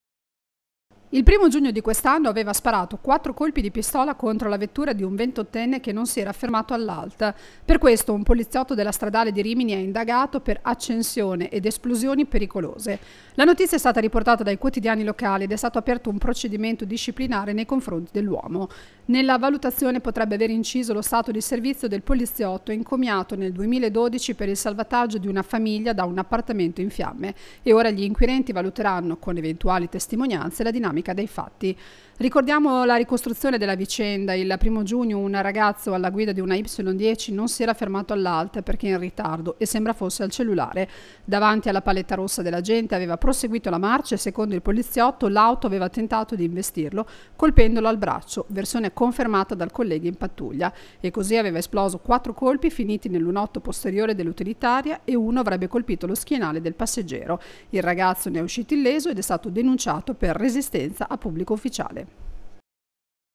RIMINI: Automobilista non si ferma, indagato il poliziotto che sparò | VIDEO
Il primo giugno di quest’anno aveva sparato quattro colpi di pistola contro la vettura di un 28enne che non si era fermato all’alt. Per questo un poliziotto della Stradale di Rimini è indagato per accensioni ed esplosioni pericolose. La notizia è stata riportata dai quotidiani locali ed è stato aperto un procedimento disciplinare nei confronti dell'uomo. Nella valutazione potrebbe aver inciso lo stato di servizio del poliziotto encomiato nel 2012 per il salvataggio di una famiglia da un appartamento in fiamme e ora gli inquirenti valuteranno, con eventuali testimonianze, la dinamica dei fatti. Ricordiamo la ricostruzione della vicenda. Il primo giugno un ragazzo, alla guida di una Y10, non si era fermato all'alt perché in ritardo e sembra fosse  al cellulare. Davanti alla paletta rossa dell'agente aveva proseguito la marcia e secondo il poliziotto l'auto aveva tentato di investirlo, colpendolo al braccio (versione confermata dal collega in pattuglia) e così aveva esploso quattro colpi finiti nel lunotto posteriore dell'utilitaria e uno avrebbe colpito lo schienale del passeggero. Il ragazzo ne è uscito illeso ed è stato denunciato per resistenza a pubblico ufficiale